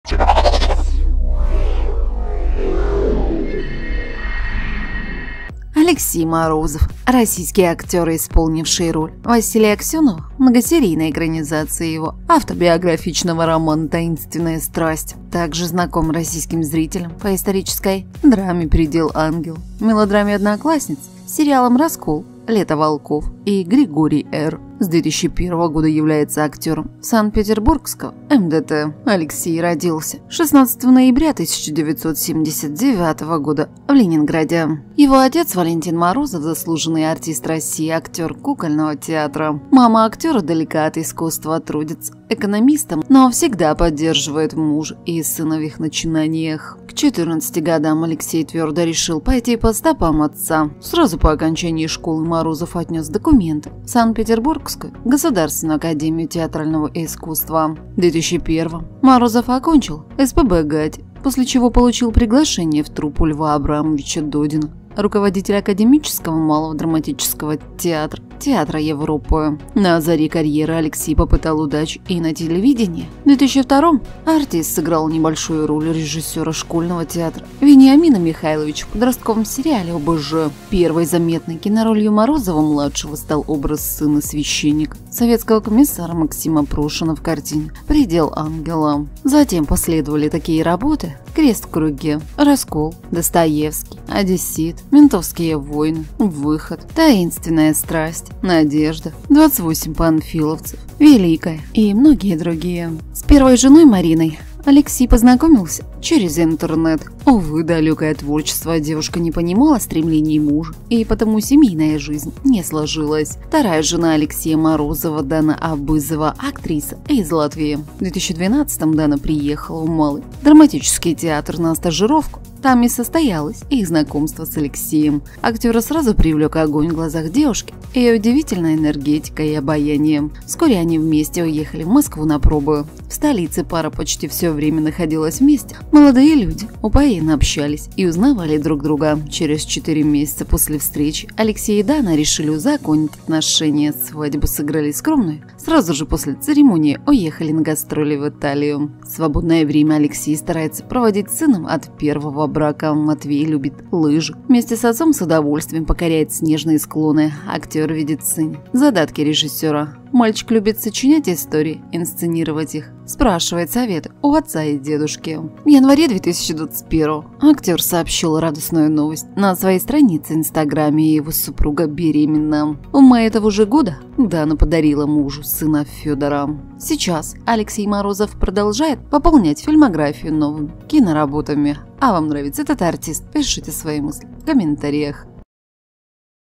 ИЗВЕСТНАЯ АКТРИСА! Кто жена и как выглядят 2 сыновей известного актера Алексея Морозова?
Алексей Валентинович Морозов – российский актер, исполнивший роль Василия Аксёнова в многосерийной экранизации его автобиографичного романа «Таинственная страсть». Также знаком российским зрителям по исторической драме «Придел ангела», мелодраме «Одноклассницы», сериалам «Раскол», «Лето волков» и «Григорий Р.». С 2001 года является актером Санкт-Петербургского МДТ...